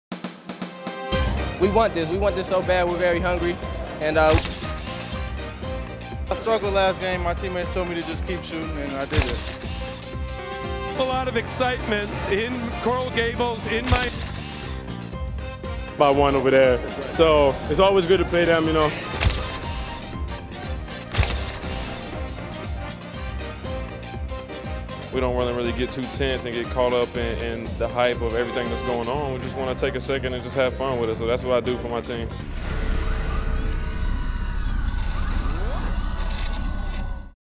Miami's Julian Gamble Photobombs ACC Tournament | ACCDigitalNetwork
During the Miami Hurricanes run to the ACC Tournament title, Julian Gamble had some fun photobombing head coach Jim Larranaga and teammates every chance he got.